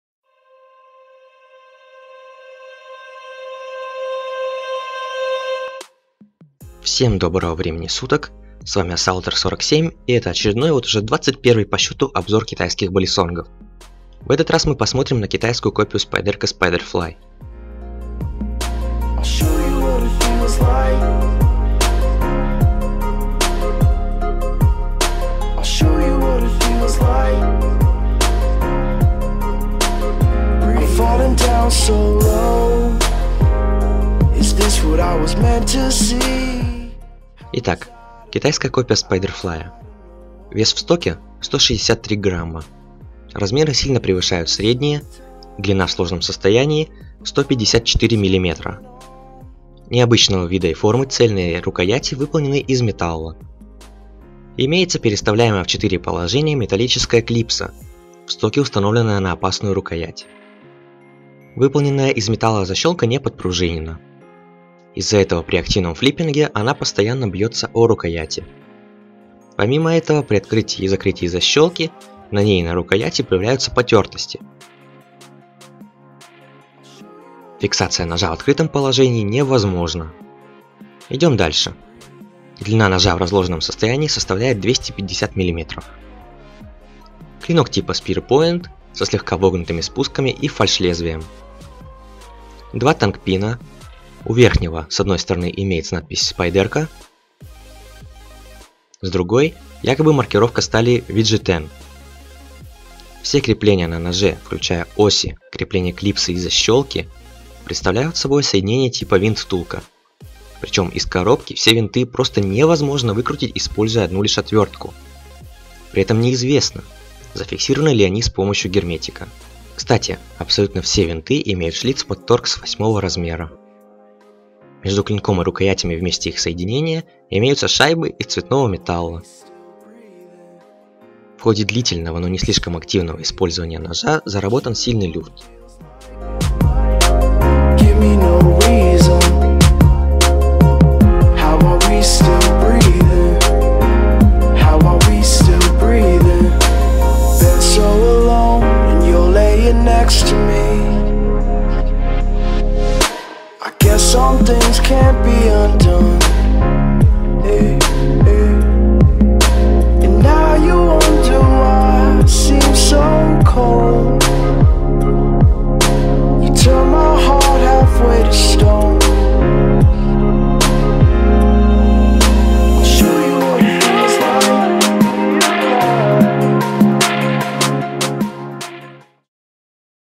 Балисонг. Обзор №21: копия Spyderco Spyderfly (Balisong Club)
Обзор очень добротной копии Spyderco Spyderfly. =D

Итоговая оценка:

Удобство флиппинга: 8/10
Качество: 8/10